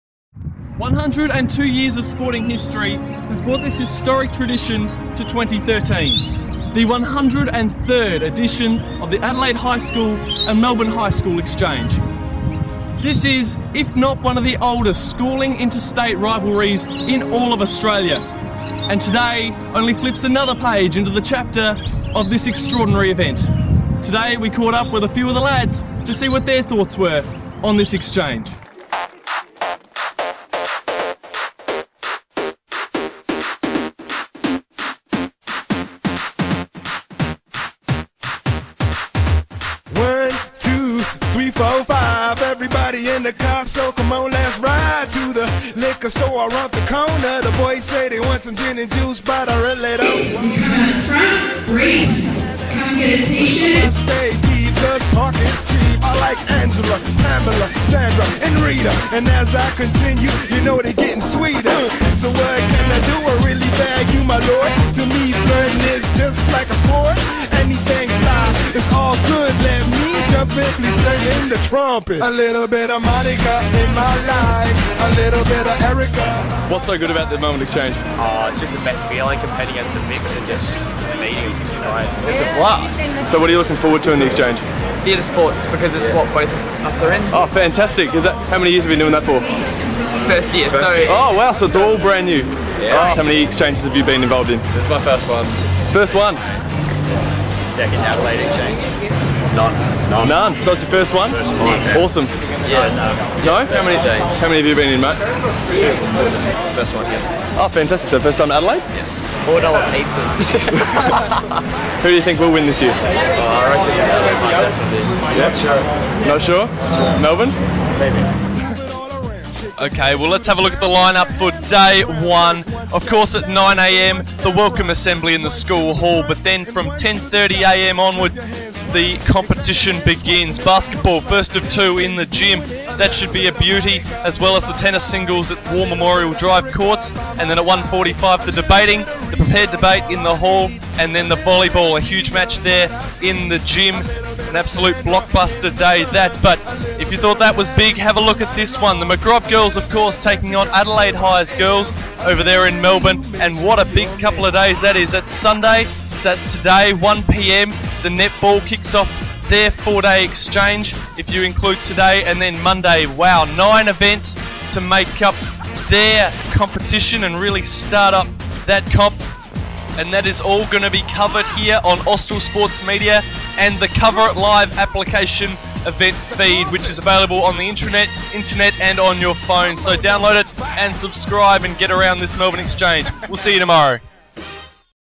2013 AHS Exchanges: Sunday Arrival and Day 1 Preview
Austral Sports Media meets some of the participants in this year's Melbourne Exchange, and has a look at the week ahead.